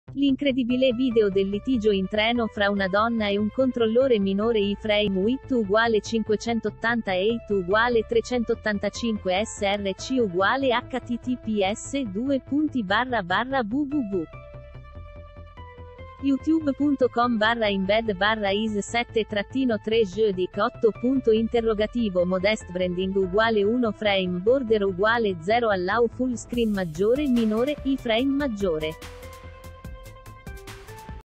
L'incredibile video del litigio in treno fra una donna e un controllore
L'incredibile video del litigio in treno fra una donna e un controllore iframe width="580" height="385" src=" frameborder="0" allowfullscreen/iframe